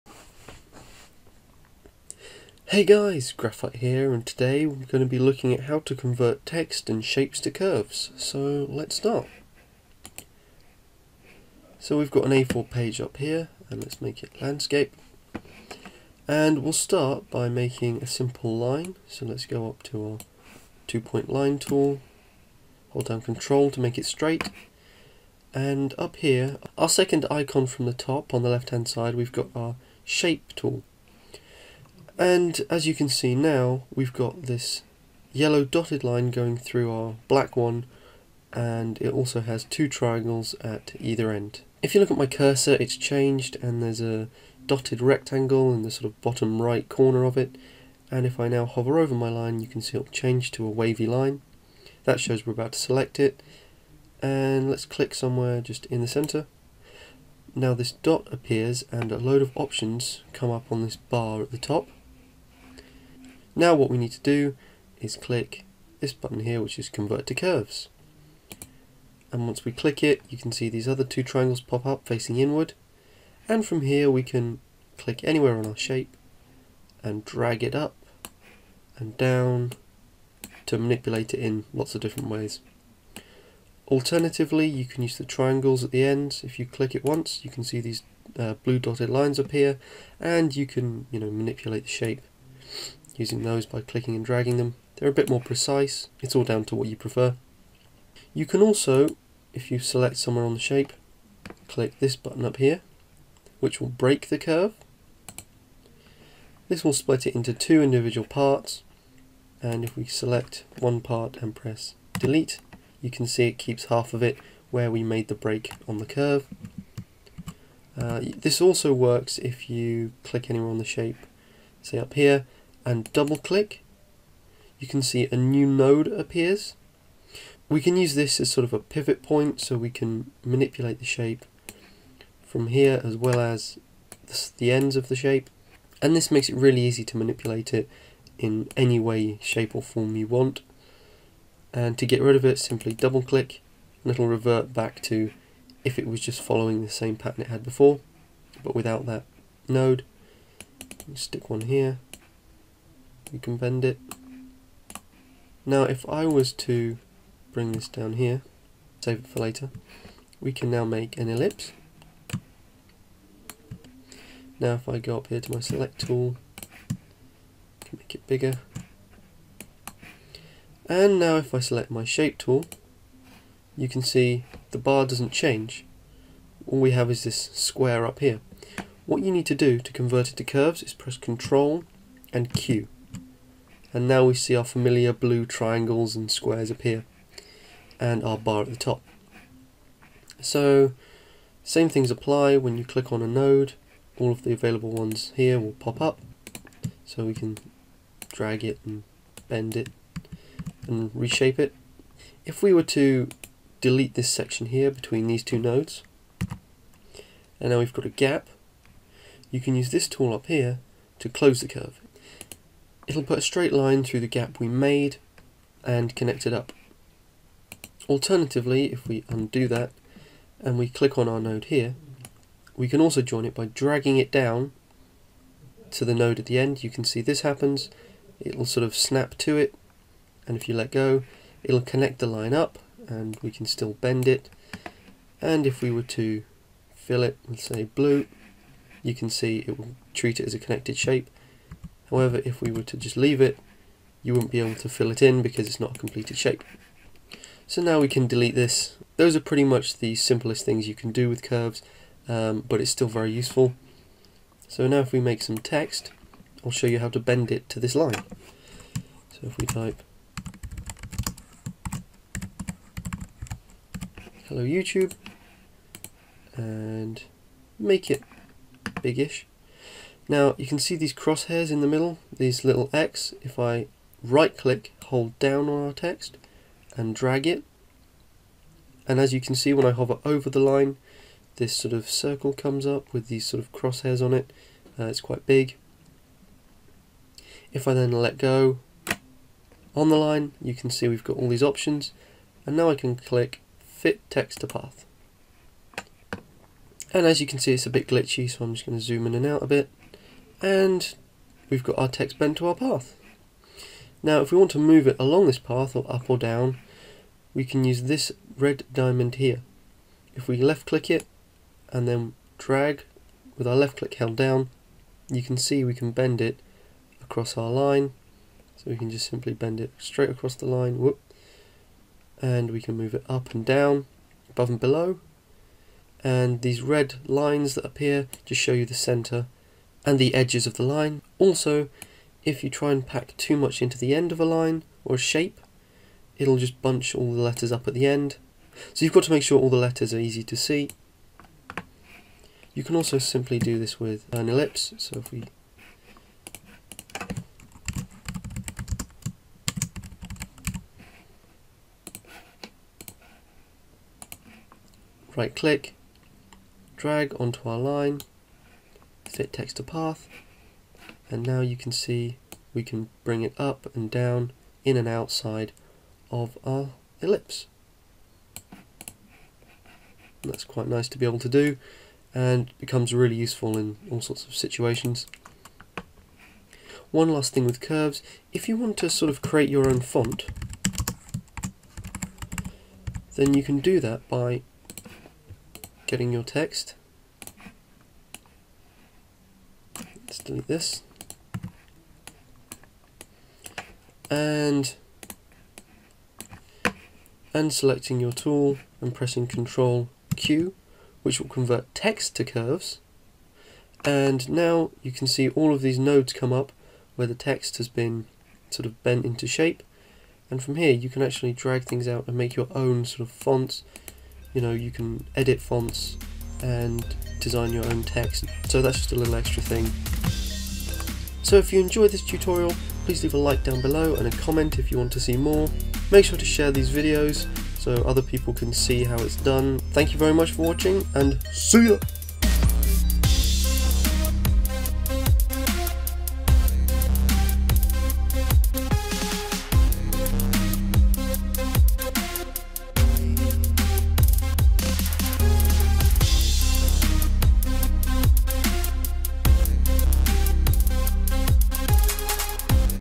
How to convert shapes & text to curves. Corel Draw X6 Tutorial
How to convert shapes and text to curves and manipulate them. Corel Draw X6 Tutorial.
Make sure to have your video quality on ~~ 720p HD ~~ or the video WILL look pixelated. Thanks!
- Graphite